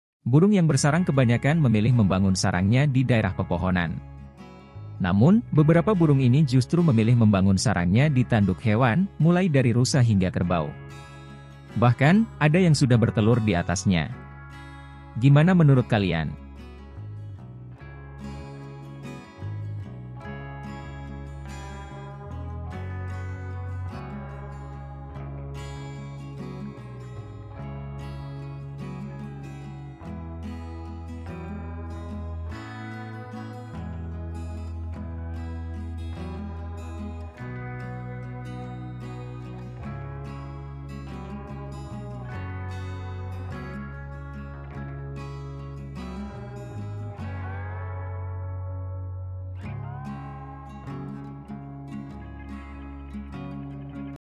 Sarang burung di tanduk hewan rusa dan kerbau
Burung yang bersarang di tanduk hewan kebanyakan memilih membangun sarangnya di daerah pepohonan. Namun, beberapa burung ini justru memilih membangun sarangnya di tanduk hewan, mulai dari rusa hingga kerbau. Bahkan, ada yang sudah bertelur di atasnya.